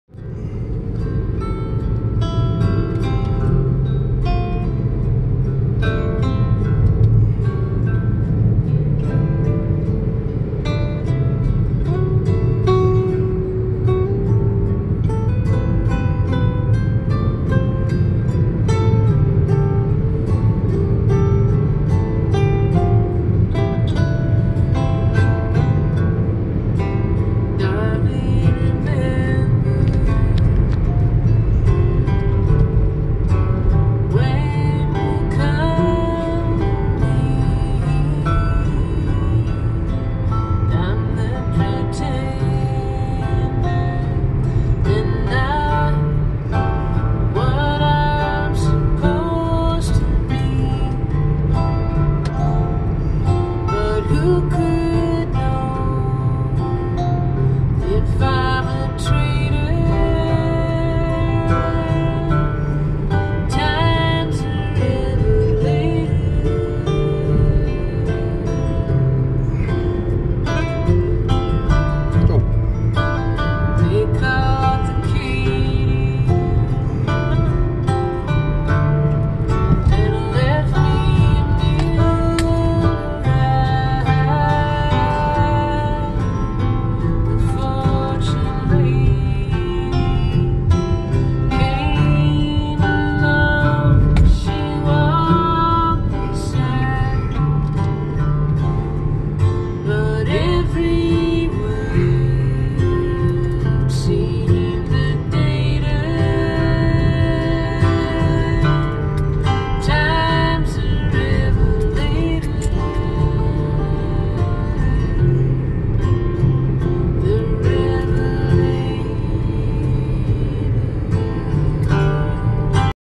Umpawaug Rd. Redding, Connecticut. September 22, 2020
Another view of Umpawaug Road, Redding CT. A lovely road and part of my daily commute.